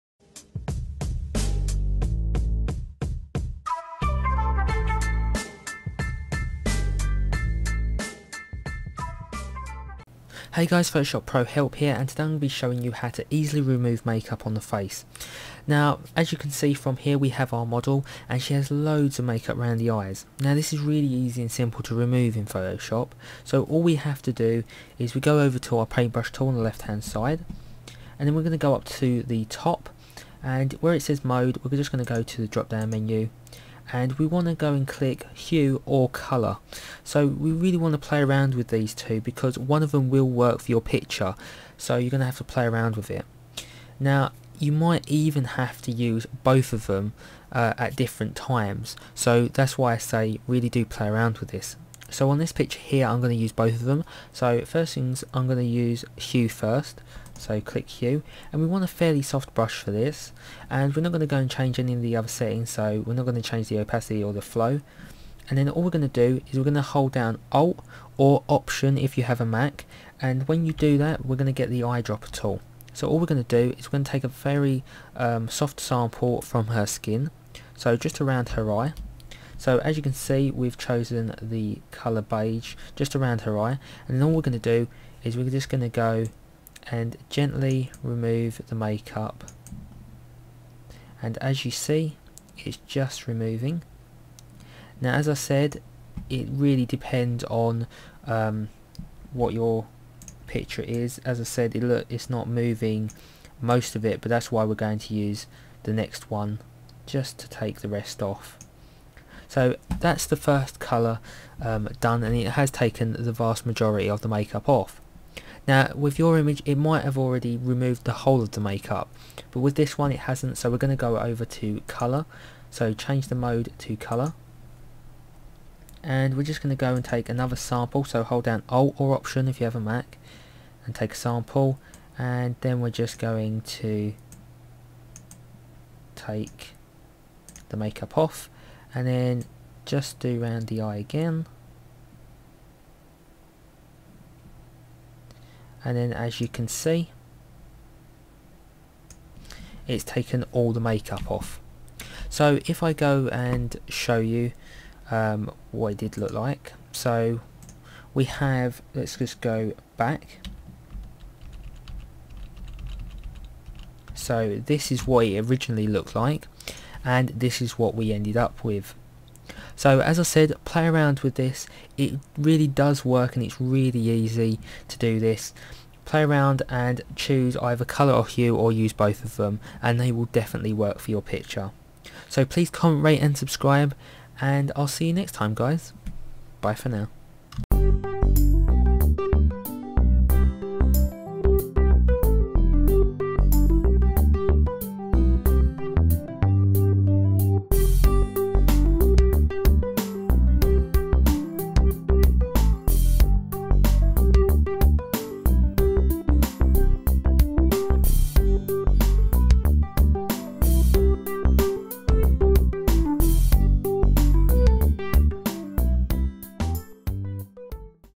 Photoshop CS6 How To Remove Make-up
Today's tutorial will teach you how to remove the make-up off your face, this is a very fast effective technique that can be used for all photo's.

So if you don't like your make-up then remove it and reapply some make-up you do like in Photoshop, never let it get you down again

The effect only takes a couple of steps and is very fast and effect to do, not only will it make your photo stylish but it will make you stand out with in the photo.

NEW T-SHIRTS!

Google Plus!